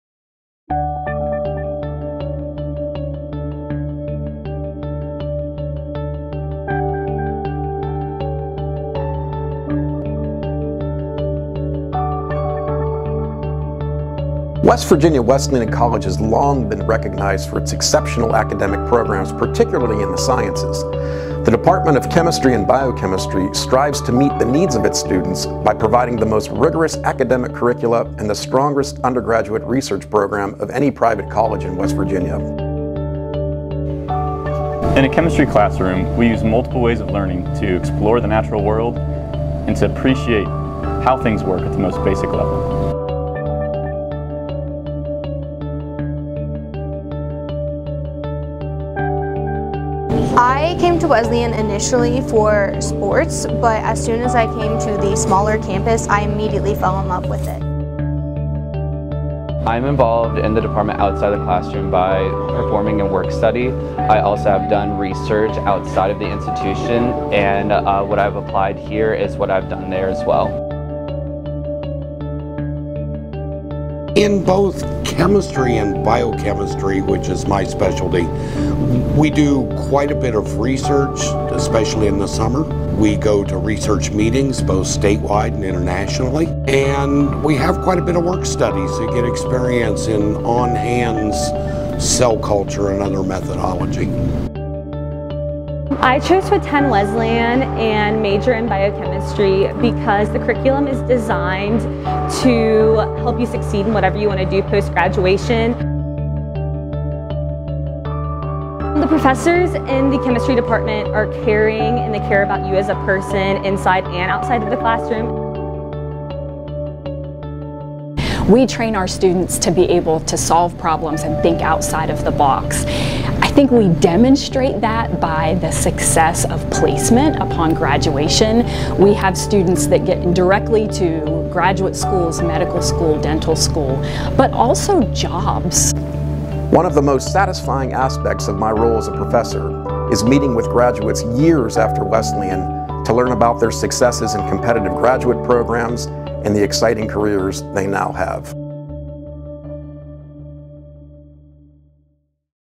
Department of Chemistry and Biochemistry Highlight
Learn more about how West Virginia Wesleyan College's Department of Chemistry and Biochemistry prepares graduates for the next step in their future.